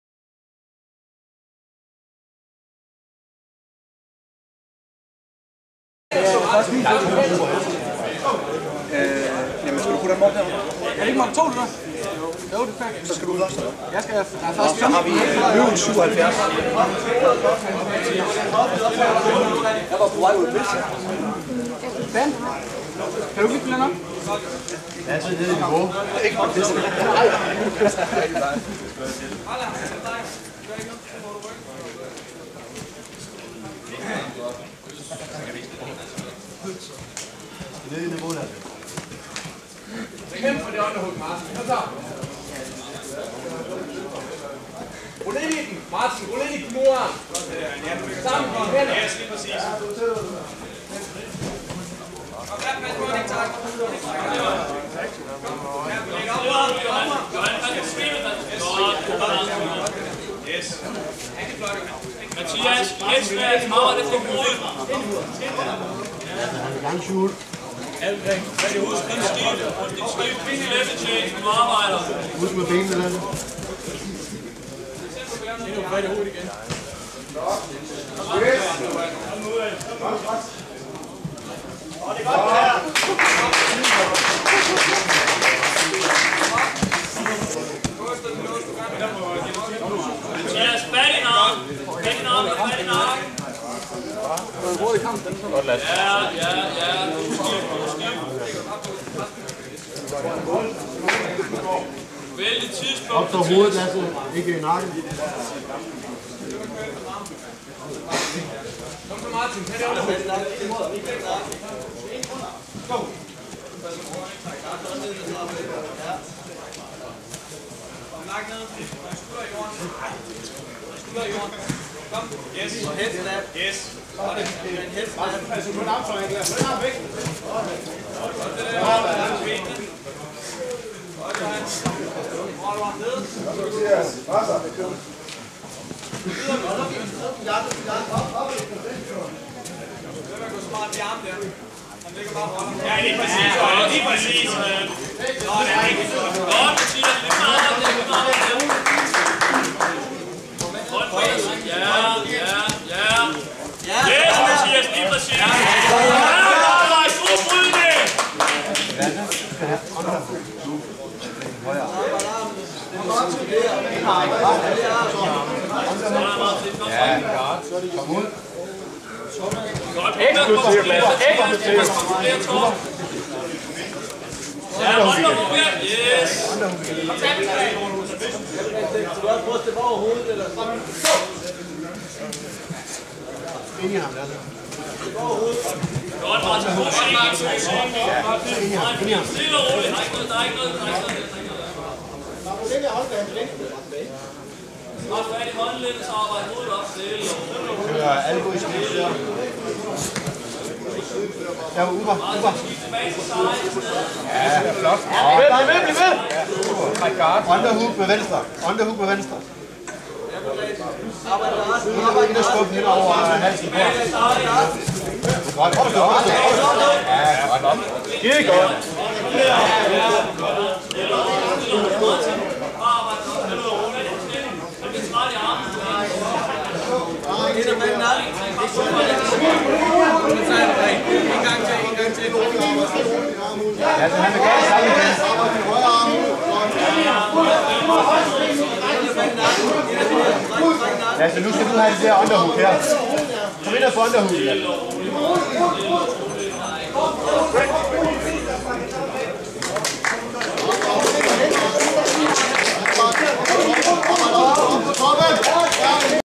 DGL 2012 1 runde F. Mathias Appelgren Fightgym.dk vs Lasse Thygesen Team Bushido
Dansk Grappling Liga 2012 1 Runde Fyn
Gruppe C Begynder Mænd -88.0 kg

Mathias Appelgren Fightgym.dk vs Lasse Thygesen Team Bushido